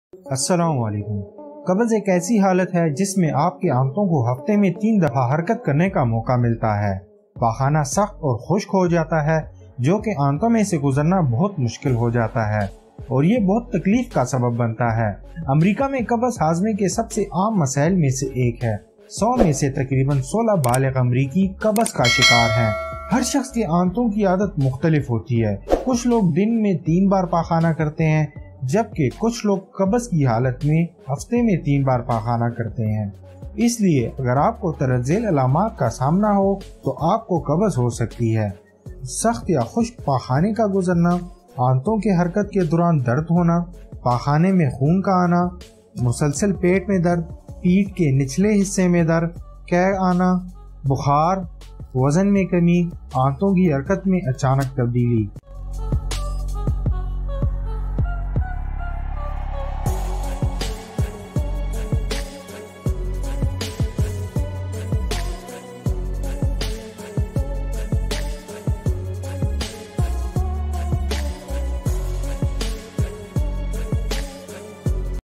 What Is Constipation | Signs Of Constipation
Assalam o Alaikum! In this video, we are discussing Constipation and the signs of constipation